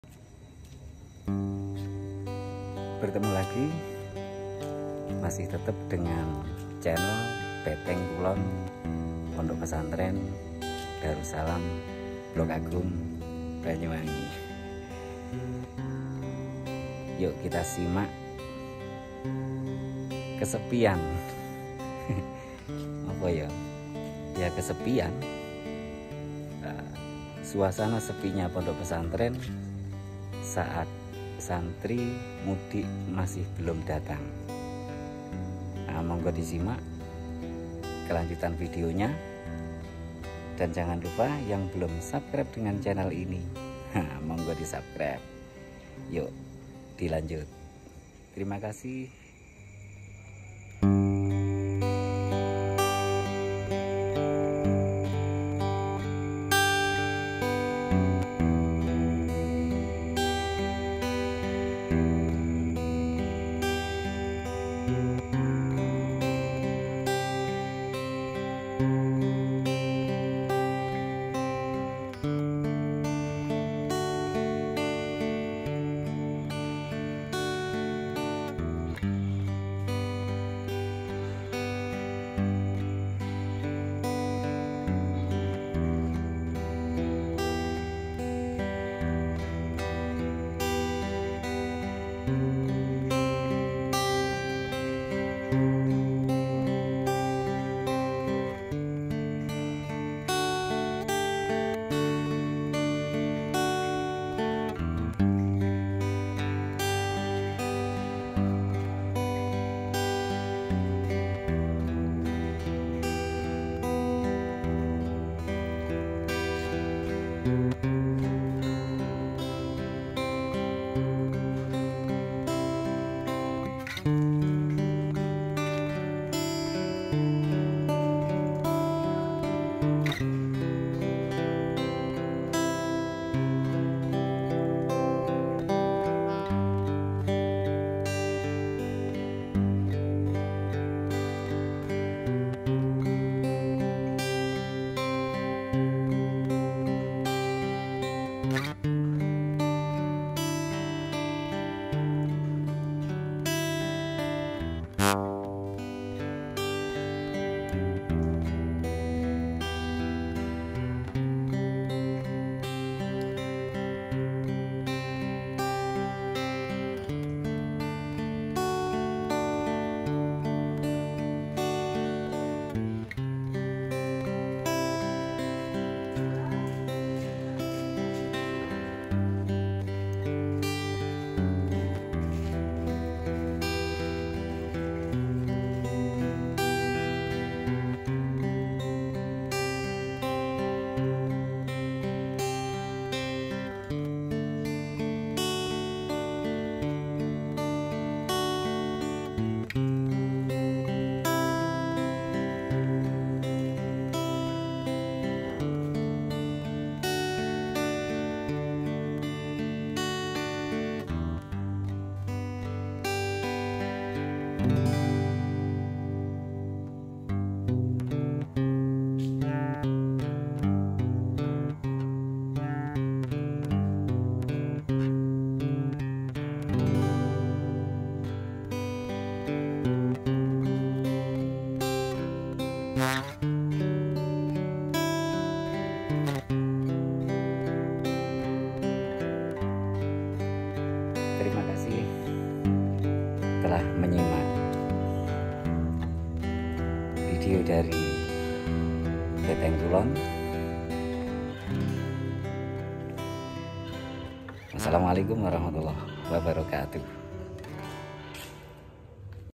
Sepi nya Pesantren
SEPI NYA PONDOK PESANTREN SAAT SANTRI MUDIK DAN BELUM KEMBALI